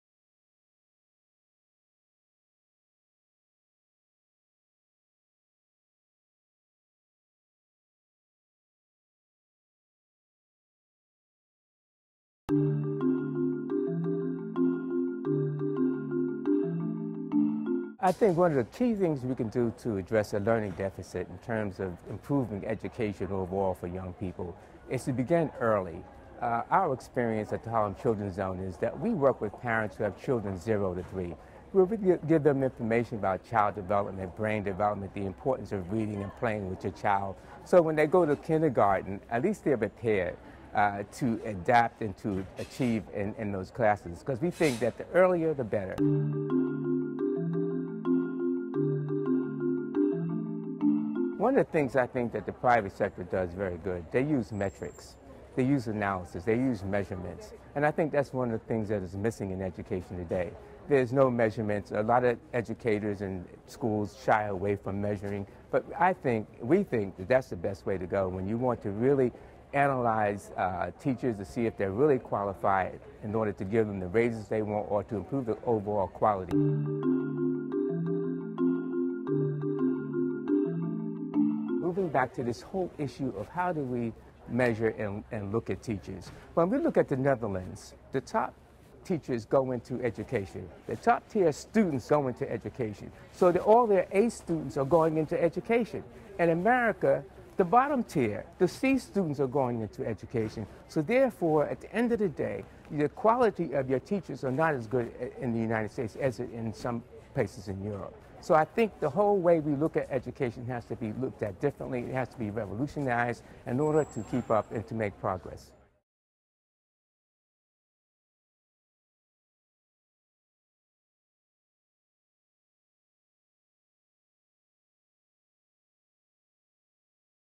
Education for All: George Khaldun, Harlem Children's Zone
United Nations, New York, 12 April 2011 - "The way we look at education has to be looked at differently, it has to be revolutionized in order to keep up and make progress" says Mr. George Khaldun, Chief Operating Officer, Harlem Children's Zone.

Mr. Khaldun answers the following questions:

What can be done to address the learning deficit?

What added value can the private sector bring to education policy-making and delivery?

Is there a role for the private sector to help close the global teacher gap?

Mr. Khaldun was interviewed prior the special event of the Economic and Social Council (ECOSOC), "Partnering with the philanthropic community to promote education for all" on 28 February 2011.

This event was one of the preparatory events for the 2011 ECOSOC Annual Ministerial Review (AMR) on "implementing the internationally agreed goals and commitments in regard to education", and was organized by the Department of Economic and Social Affairs (UNDESA), in collaboration with the United Nations Children's Fund (UNICEF), United Nations Educational, Scientific and Cultural Organization (UNESCO), the United Nations Office for Partnerships (UNOP), and the Committee Encouraging Corporate Philanthrophy (CECP).

The key objective of the special event was to increase focus on issues central to the achievement of education for all, through:

(i) broadening of the range of stakeholders involved in the work of ECOSOC, as well as increasing the awareness amongst the philanthropic community on the progress made and the challenges faced in achieving the internationally agreed goals and commitments on education;
(ii) promoting concrete initiatives by the philanthropic community and initiating new partnerships that would accelerate progress in reaching the international goals and targets related to education;
(iii) expanding multi-stakeholder engagement in the international community's efforts to help advance the progress on the two focus areas mentioned above.